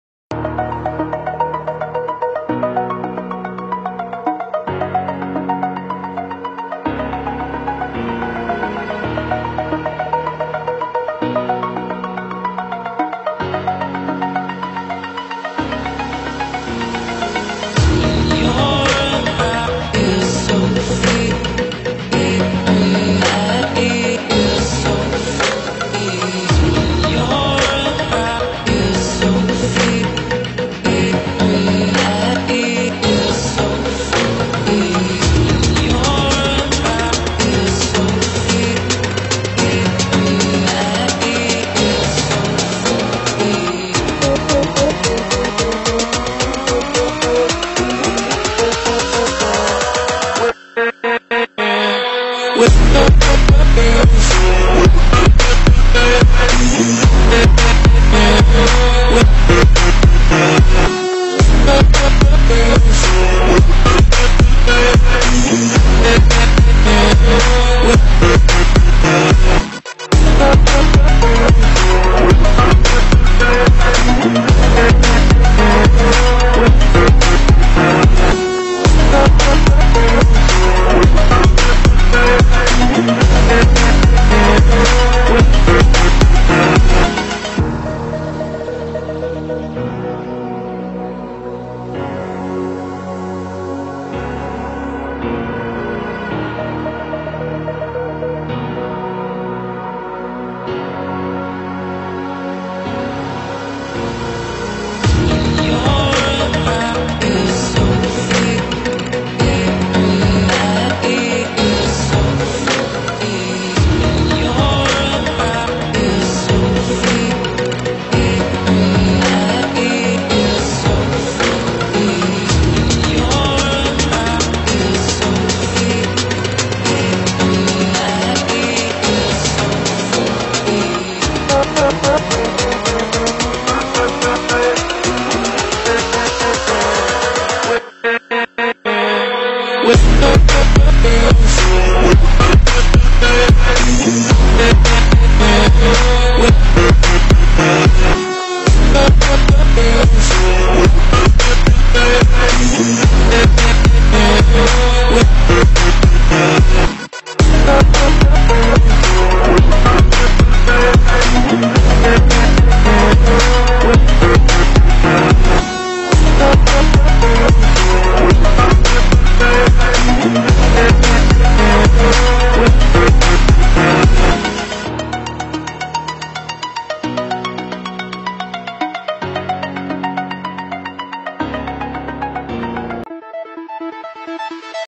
Butterfly in Phone Prank App 2018|Best Live Wallpapers For Android Mobiles
Butterfly in Phone Prank App is animated Beautiful various kinds of Butterflies.

Hi! There,

This is an informative Video Only.

Video Information

1.App Name:Butterfly in Phone Prank

2.Availability: Google Play Store

3.App Size:4.6MB

4.Platform: Android

5.Usage & Features:

-Animated Butterfly live wallpaper
-set Number of Butterflies
-Delay time settings